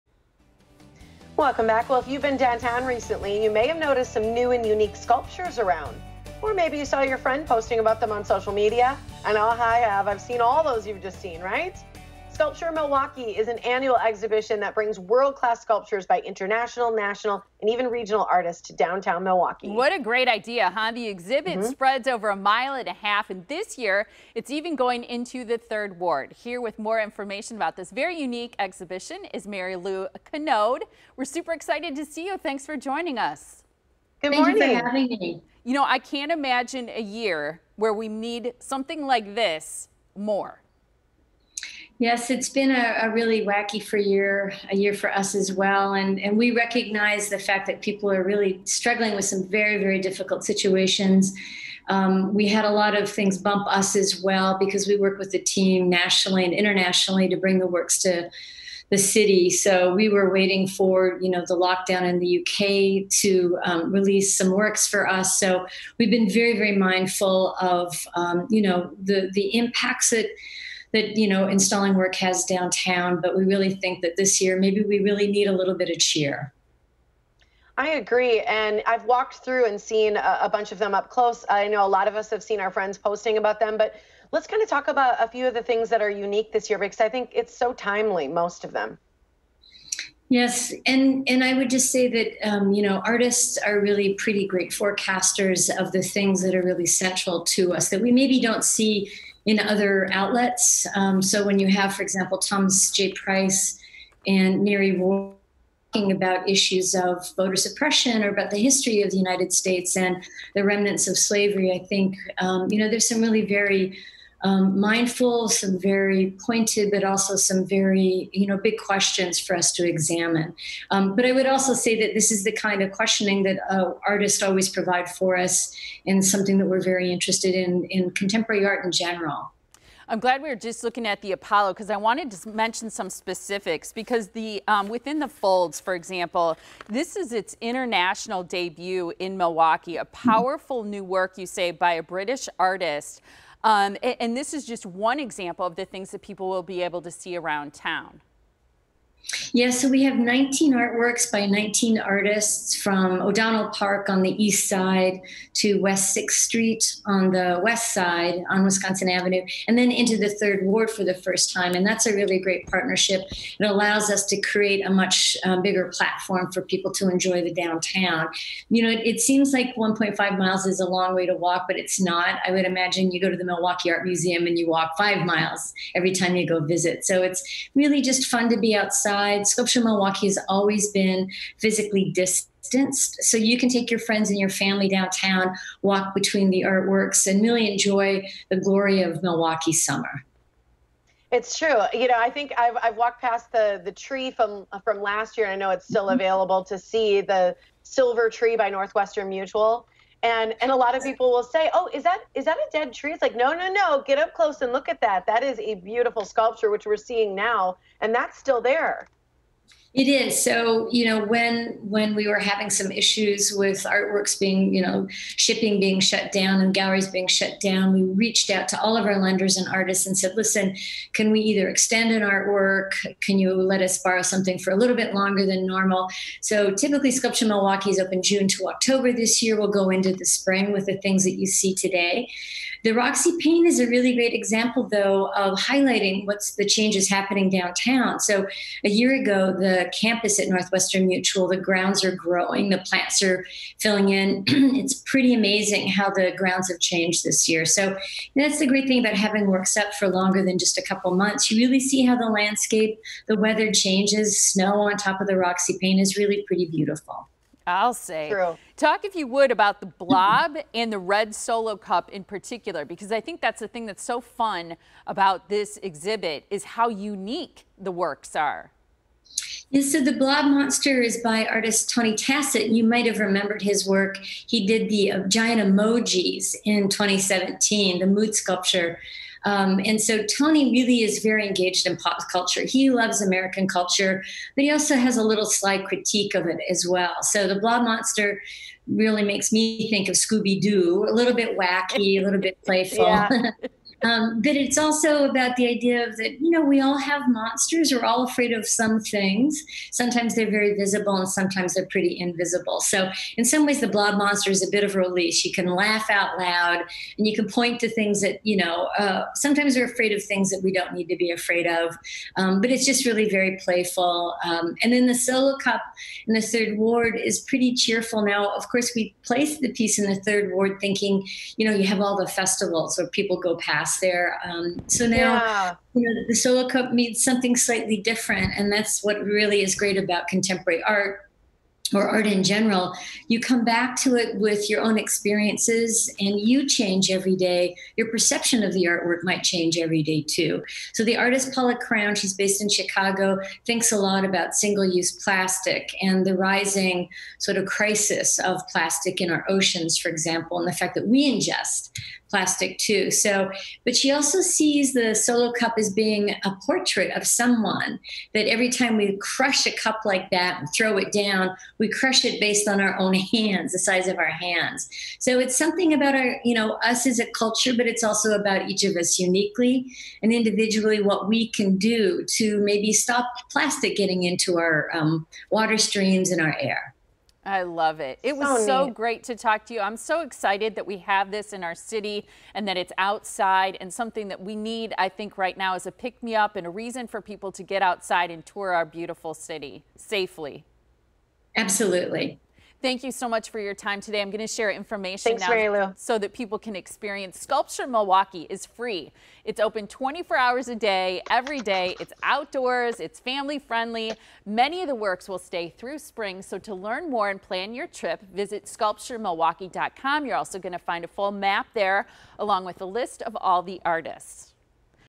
World Class Art in Milwaukee!
Couldn’t your daily life use more world class sculptures? That’s the idea behind Sculpture Milwaukee, an annual exhibition that brings 20 world-class sculptures by world-class artists to the heart of downtown, and new this year into the Historic Third Ward. Sculpture Milwaukee is free and open 24 hours a day, we’re outdoors, family-friendly and open everyday. This year many of the works will stay through the spring, so you can see how they change with the seasons. Sculpture Milwaukee is a hybrid public-private partnership, and part of a new wave of curated public exhibitions that bring such great works to our street, giving Milwaukee one of the most beautiful cultural landscapes in the country. Marilu Knode - Director of Curatorial Affairs and Education with Sculpture Milwaukee will preview some of the fabulous art that you can see anytime!